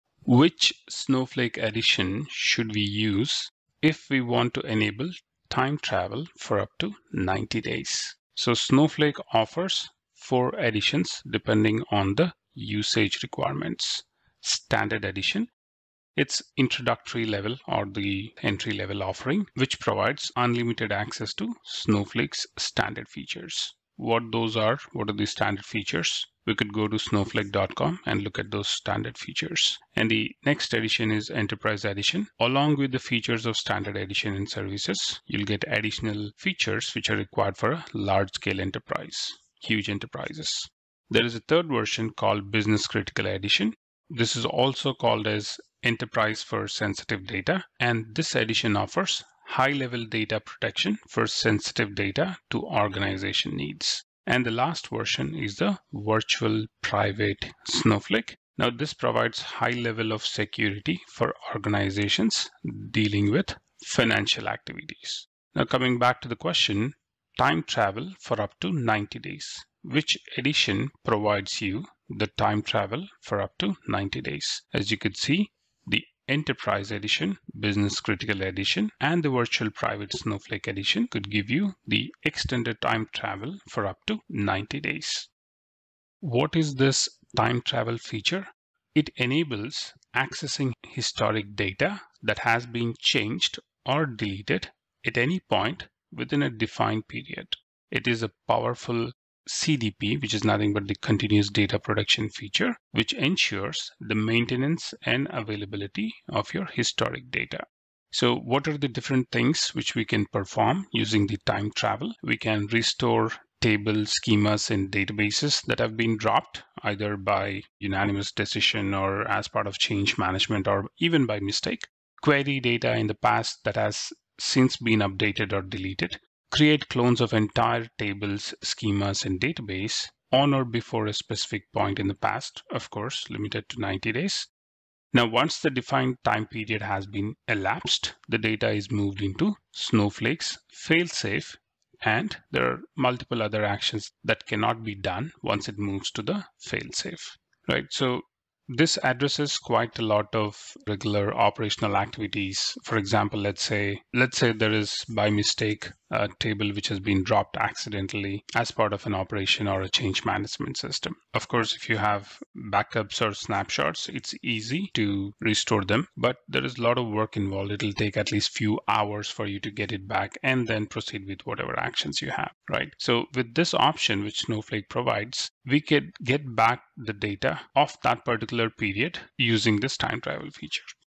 Time travel in Snowflake
Enterprise Edition, Business Critical Edition and VPS Edition have the option to enable time travel for up to 90 days.